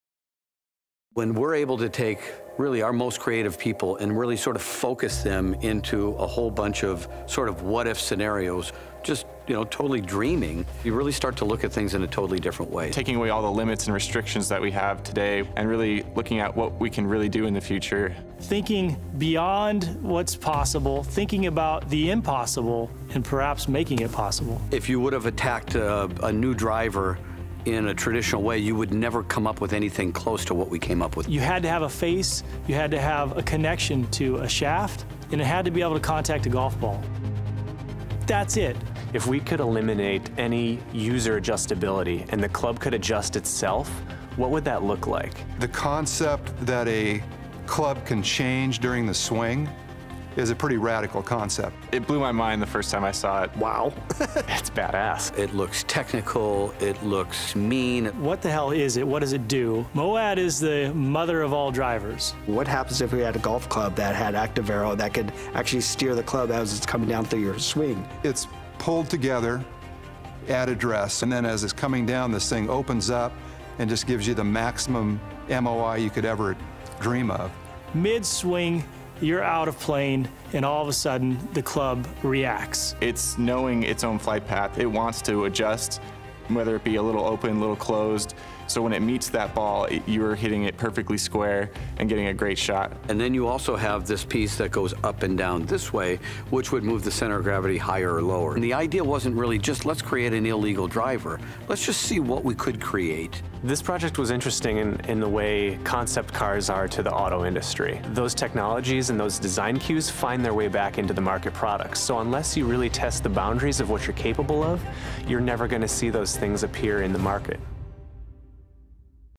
TaylorMade - MOAD by Scratch Media
As car companies do with futuristic concept cars so does TaylorMade for the golf industry as depicted in this video featuring a truly remarkable creation called: “The Mother of All Drivers.”